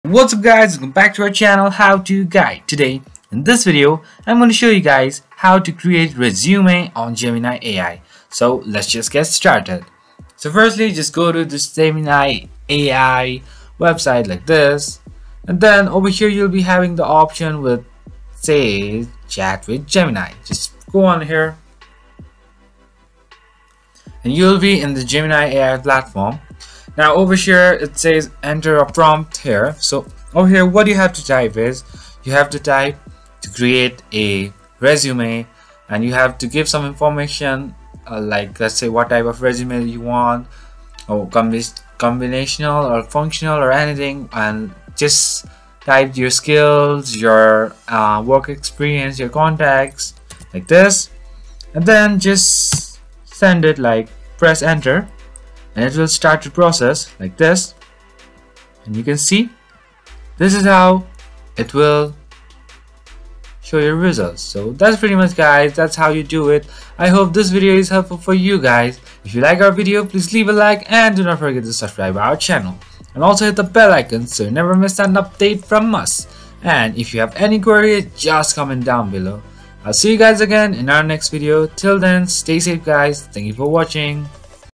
How to Create Resume on Gemini AI 2024
Learn how to craft a standout resume effortlessly with Gemini AI, the innovative platform revolutionizing the job application process. In this tutorial, discover how to harness the power of artificial intelligence to create a professional resume that highlights your skills, experience, and achievements effectively. Say goodbye to the hassle of formatting and wording – with Gemini AI, you can streamline the resume creation process and increase your chances of landing your dream job. Watch now and take the first step towards a brighter career future!